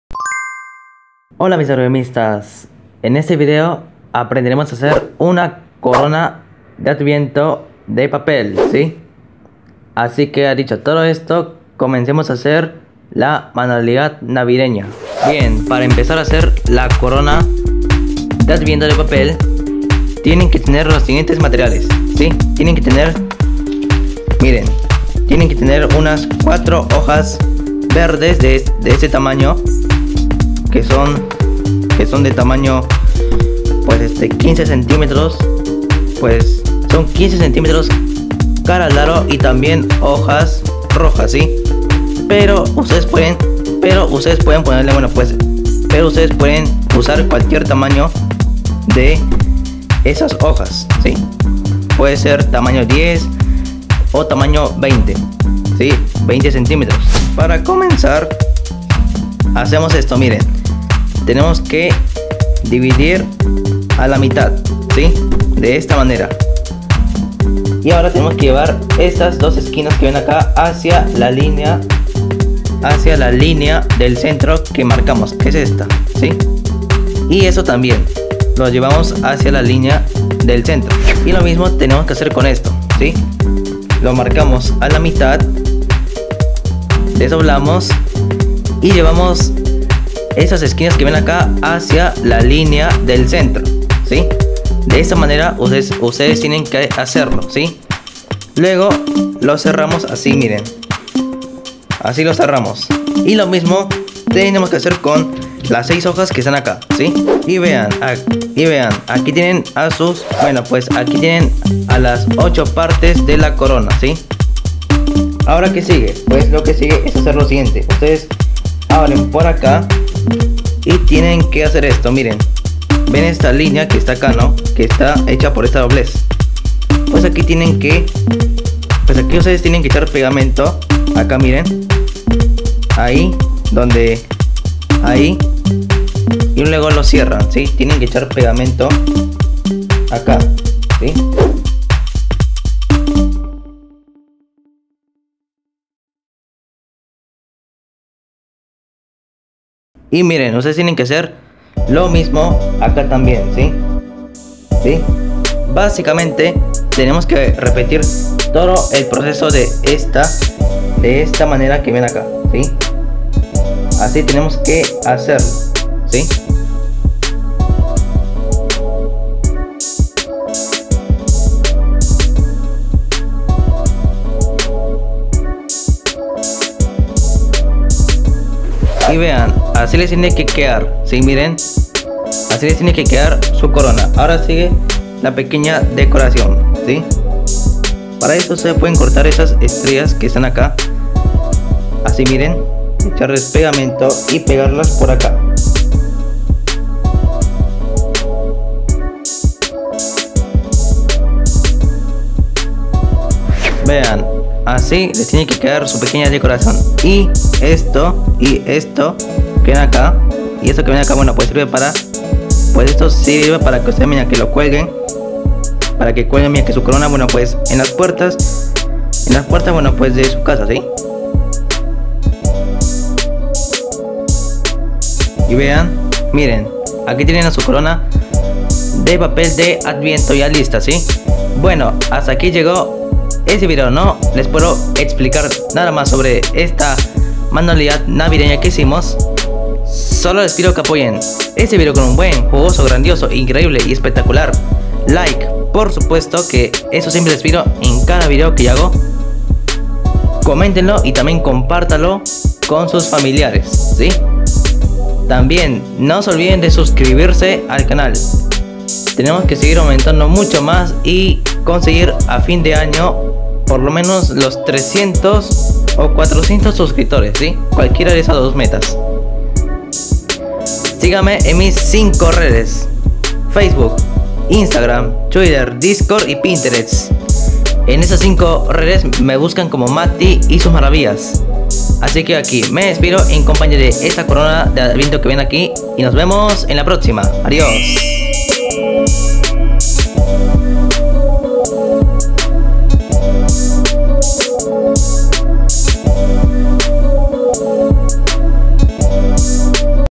Corona de Adviento Tutorial de Mati 🎄🎄 / Advent Wreath Tutorial by Mati 🎄🎄
Hola a todos, en este aprenderemos a hacer una corona de Adviento de papel, espero de que les encante mucho este video y gracias por su apoyo al canal. Muchas gracias mis Origamistas... 💖💖💕💕
Hello everyone, in this we will learn how to make a paper Advent wreath, I hope you love this video and thank you for your support of the channel. Thank you very much my Origamists ... 💖💖💕💕